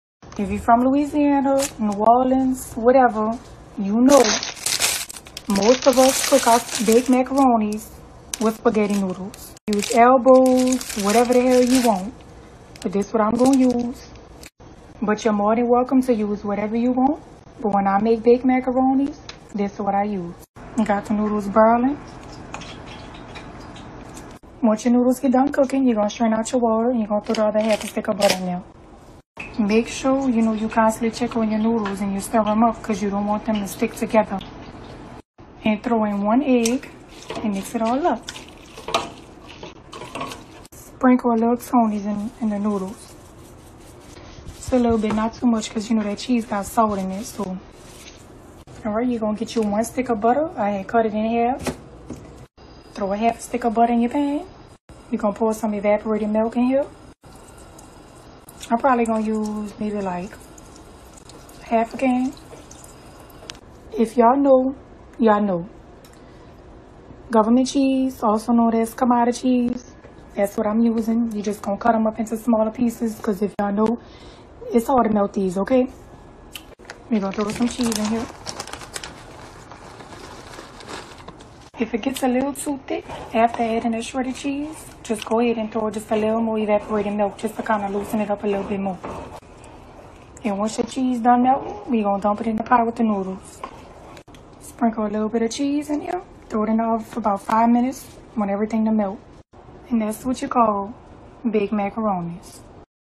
BREAKING NEANDERTHAL NEWS!!! 🙄🤢🤢🤢🤮🤮🤮👎🏿
Karen Attempts To Cook "Mac And Cheese" 🙄🤢🤢🤢🤮🤮🤮👎🏿